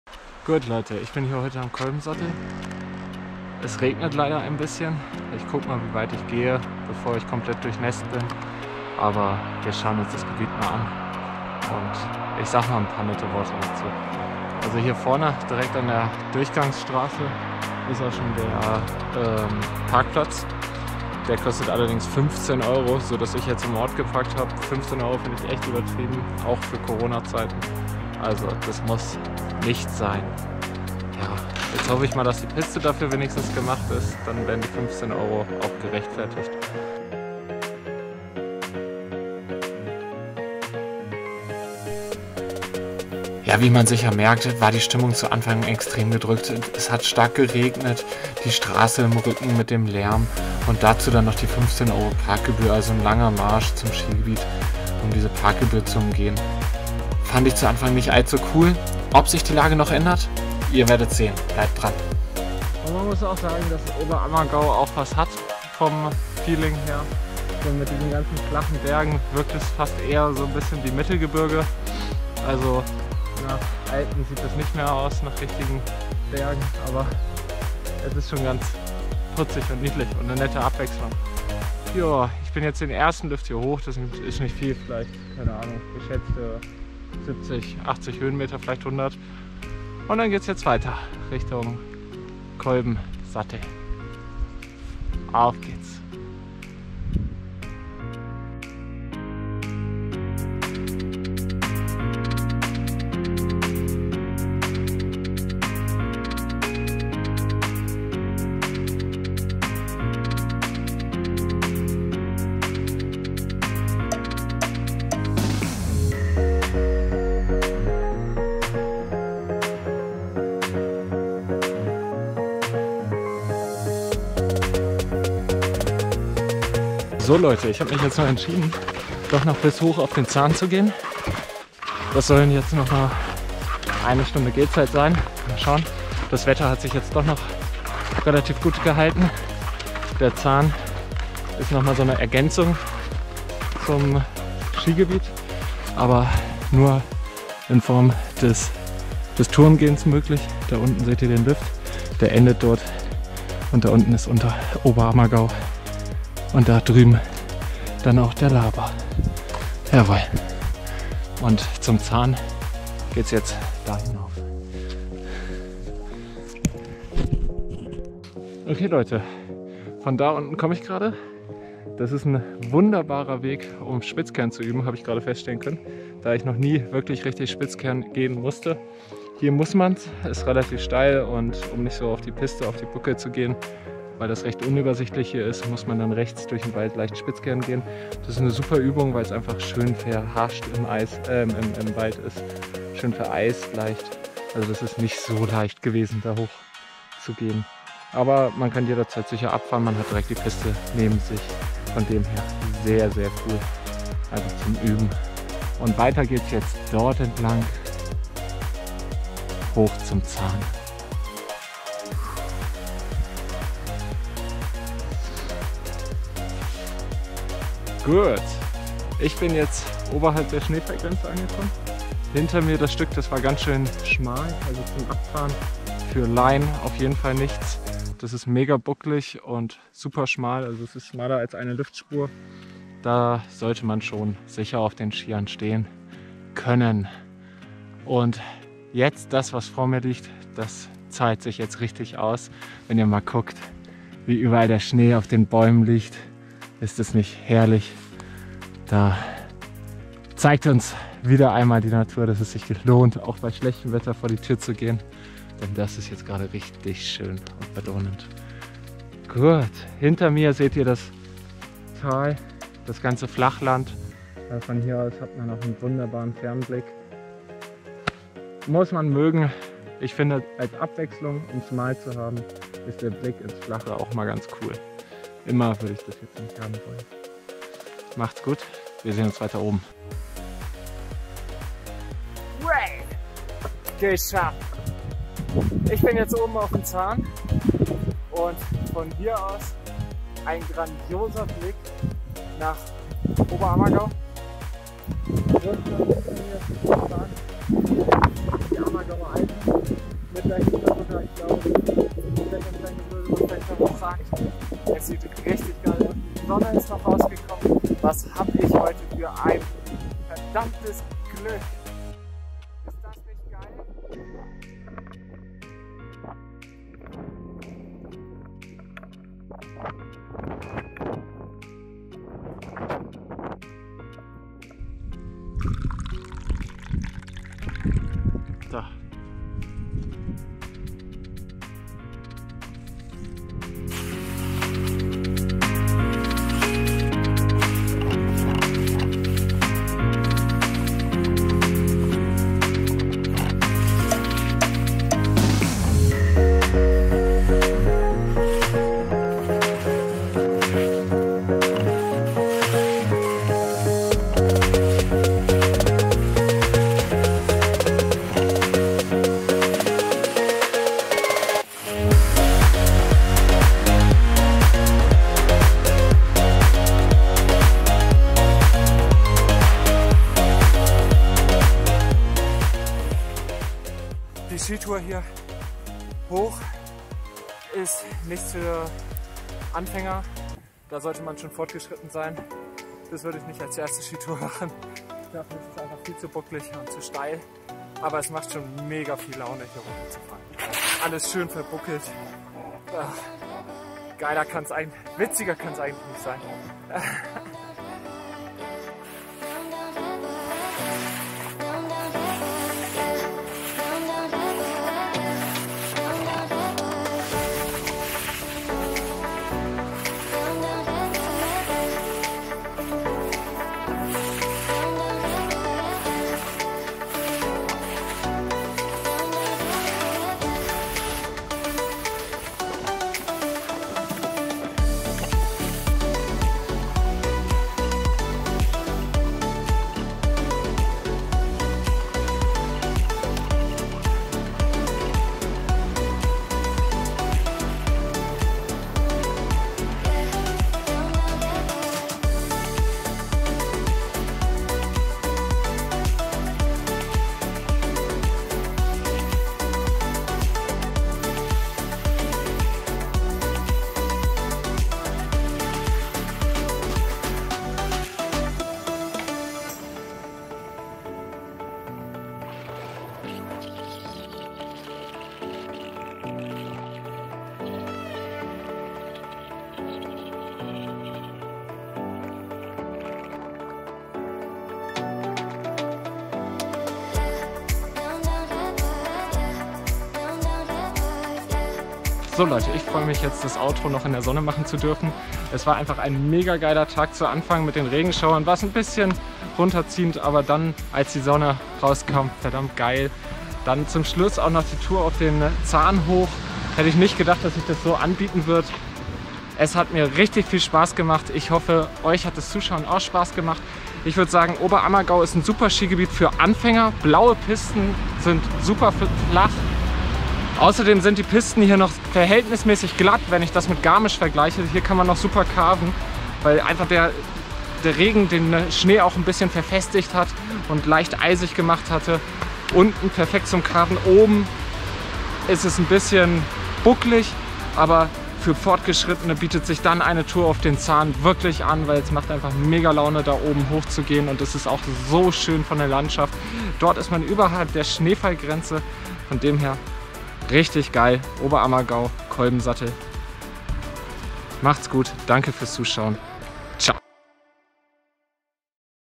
Skitour über den Kolbensattel bis zum Zahn in Oberammergau
Nur weils nach schlechtem Wetter🌧 ausschaut, kein Grund zuhause zu bleiben! ☀️
Ich bin an diesem Tag bei Dauerregen los gegangen und bei strahlendem Sonnenschein oben am Zahn angekommen.
Das kolbensattelskigebiet ist ein wunderbar geeignetes Anfängerskigebiet, mit traumhaften blauen Pisten! ⛷🎿